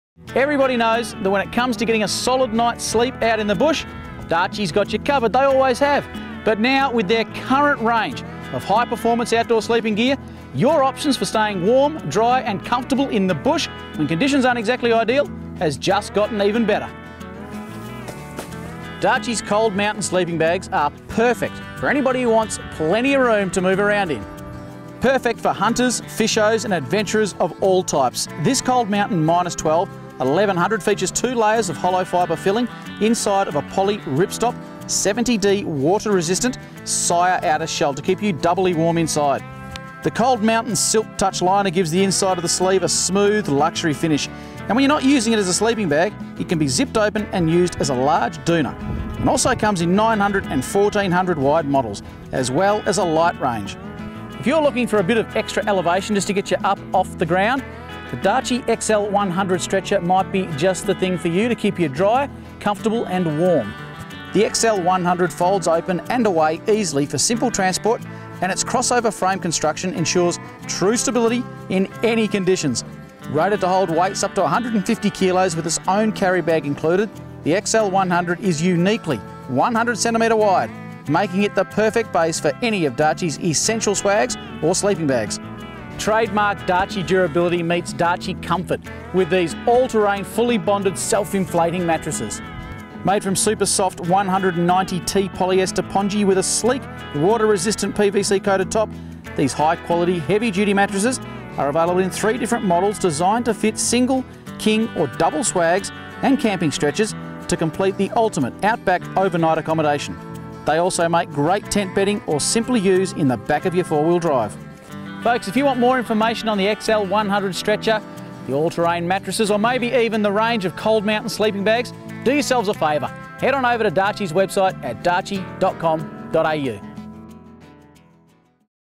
Sleeping Gear | DARCHE
When it comes to sleeping comfortably outside, Darche has you covered. Our range of high-performance sleeping gear gives you more options than ever before, for staying warm and snug even when conditions aren't cooperating. From the Cold Mountain and Cold Mountain LITE sleeping bags, to the XL100 stretcher and All Terrain self-inflating mattresses, our gear features trademark Darche durability and comfort.